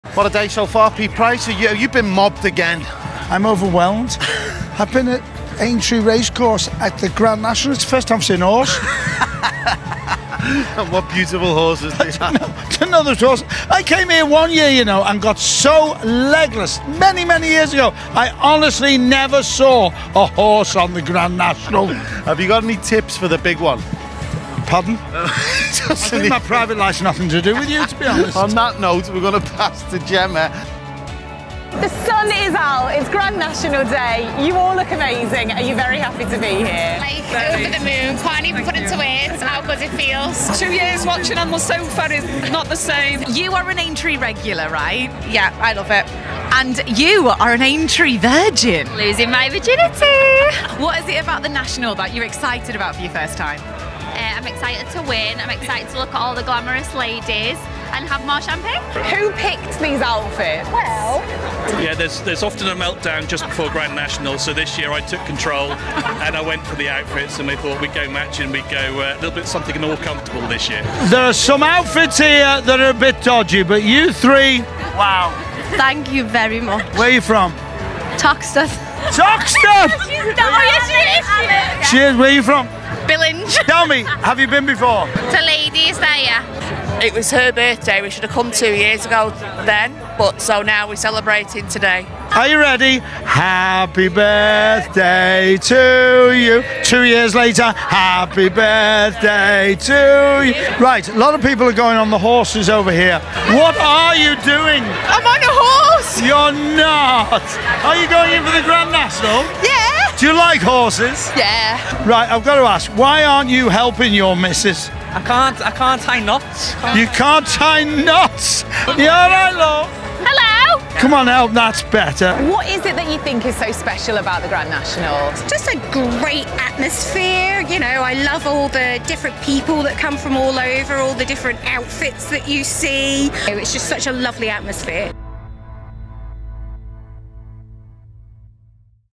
We speak to more racegoers ahead of the Grand National race | The Guide Liverpool
We speak to more racegoers at Aintree Racecourse ahead of the Randox Grand National 2022 race at 5:15pm.

There's birthday wishes, Pete telling people off and of course, more lovely outfits!